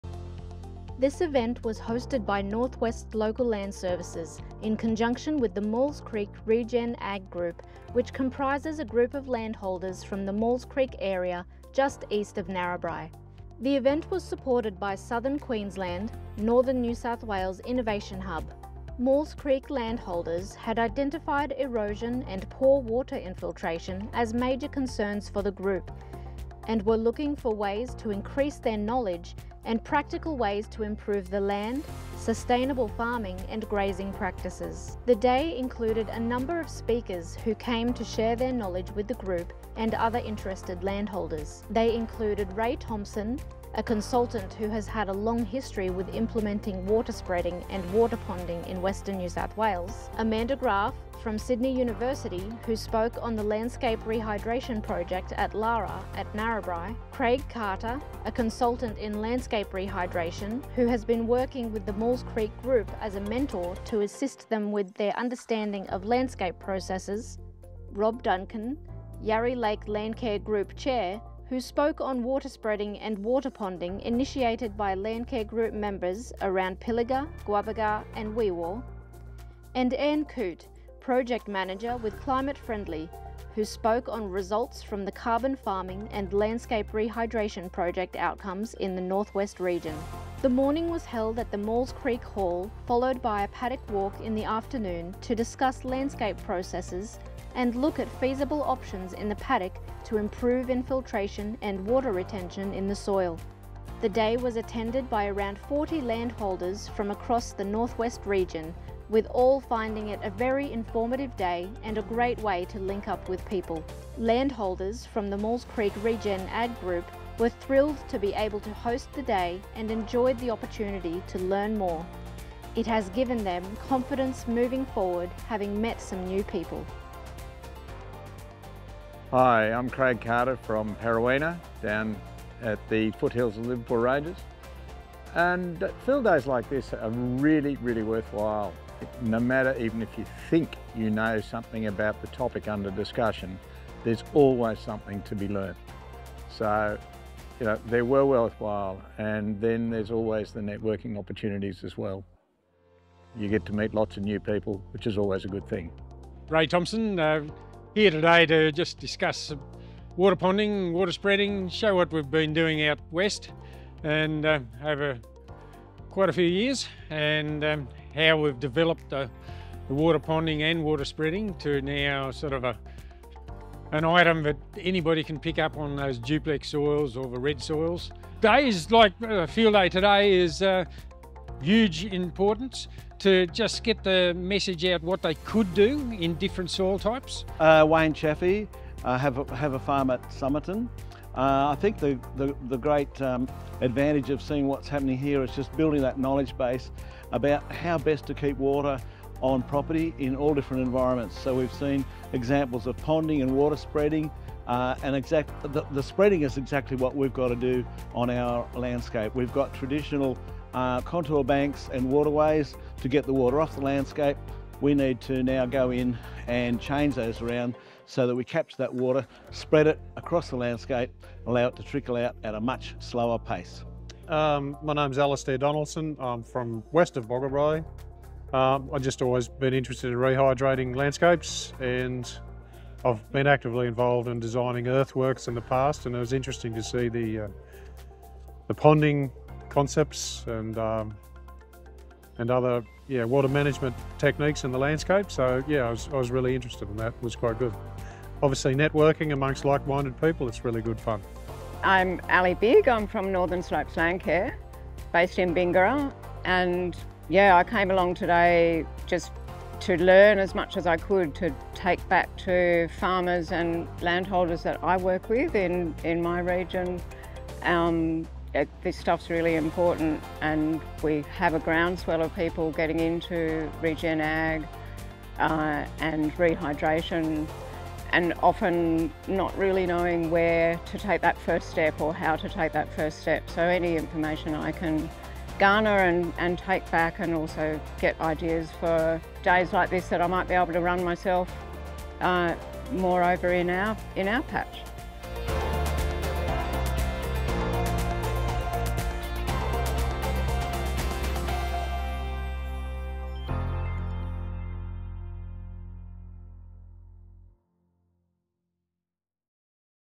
Maules Creek Regen Ag Group Landscape Rehydration Field Day Overview
The Maules Creek Regen Ag Group formed in early 2022 following concerns from four local farmers around erosion and water infiltration.  The group is located east of Narrabri and now consist of around 20 members who represent the varied range of farming activities in the area, from broadacre cropping to cotton and livestock.  The group initially contracted a mentor to help guide the group and look at on-farm options. A recent field day on Landscape Rehydration saw around 40 landholders attend.